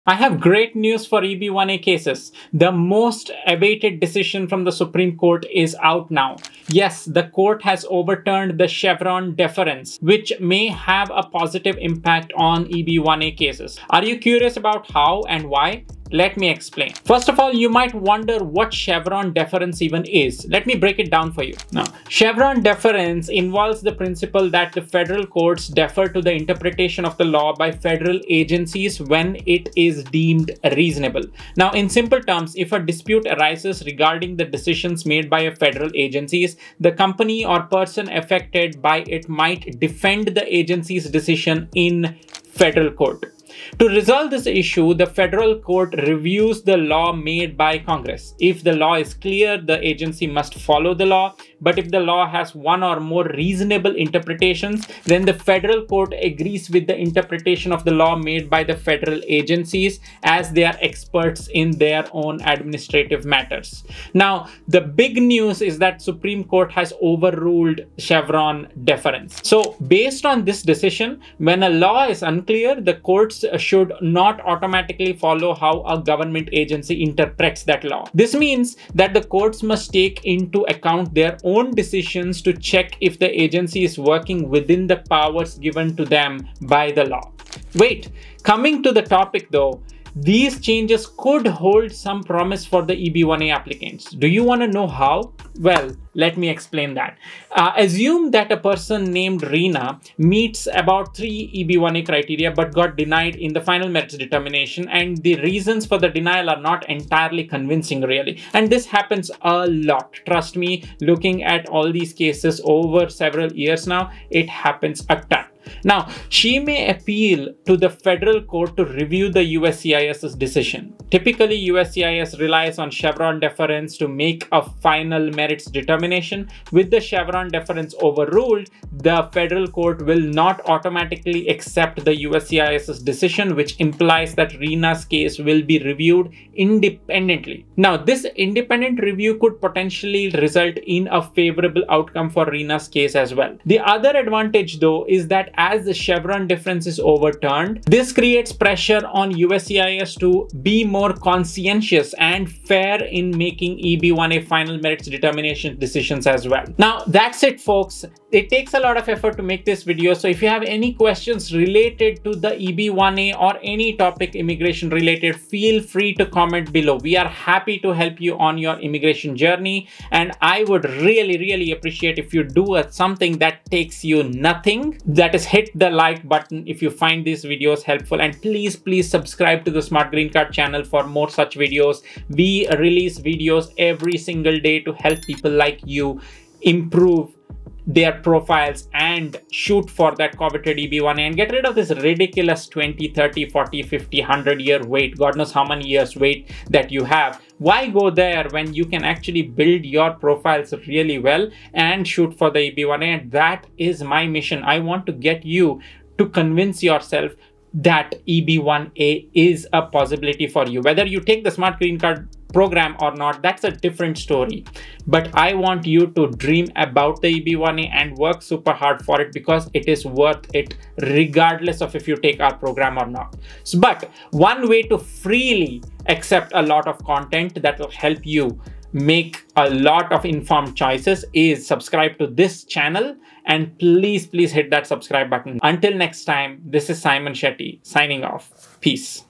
Chevron Deference is Dead! Improving the Chances of EB1A Approval? || Smart Green Card
Did you know? The Chevron doctrine has been overturned, potentially benefiting EB1A cases. In this video, we explain Chevron deference and how its reversal can help EB1A applicants. Watch this insightful video now to stay informed!

Comment what topics you want to be discussed in future videos!✨

If you are looking to take the EB1A Green Card route, here's something that will help you: